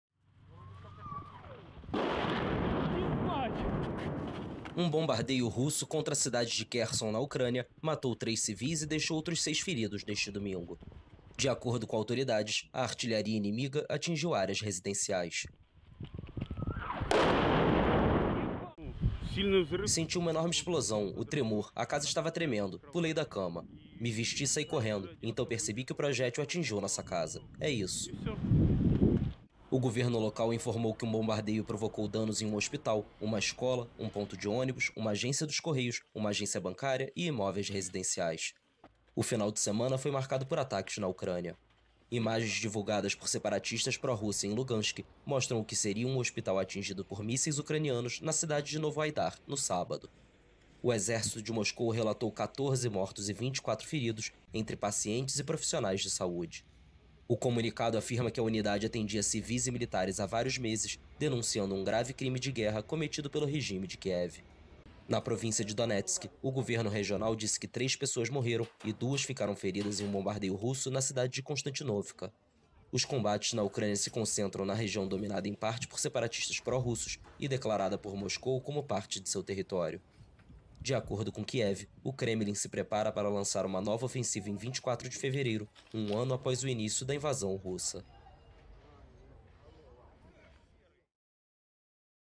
Bombardeios e mortes marcam final de semana na Ucrânia | AFP
Final de semana de intensos bombardeiros na Ucrânia, deixando vários mortos. Neste domingo, autoridades relataram um ataque contra a cidade de Kherson. Um dia antes, mísseis atingiram Konstantinovka e autoridades pró-Rússia denunciaram uma ofensiva contra um hospital em Novoaidar.